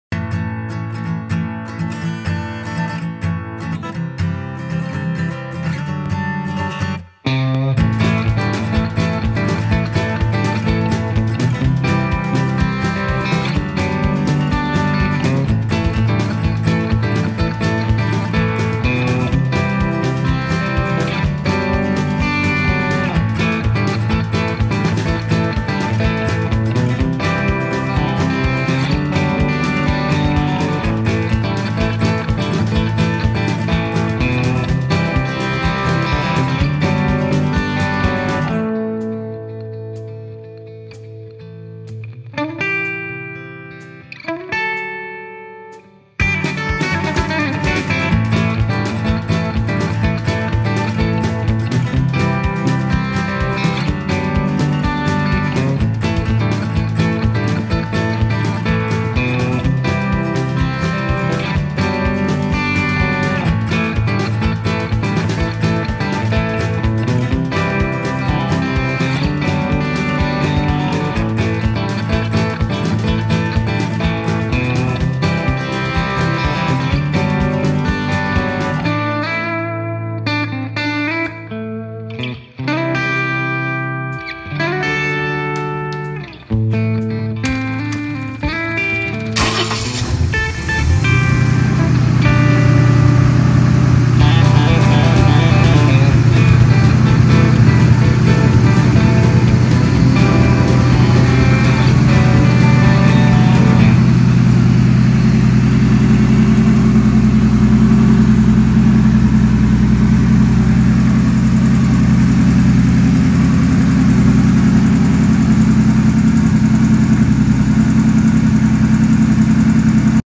ED FALCON POWERED GENERATOR.
Winter 2019 project. 4.0 ED Falcon engine connected to 10kva generator to power the shop during power outages.engine now has 'Aussie Speed'  manifold and holley 4 barrel.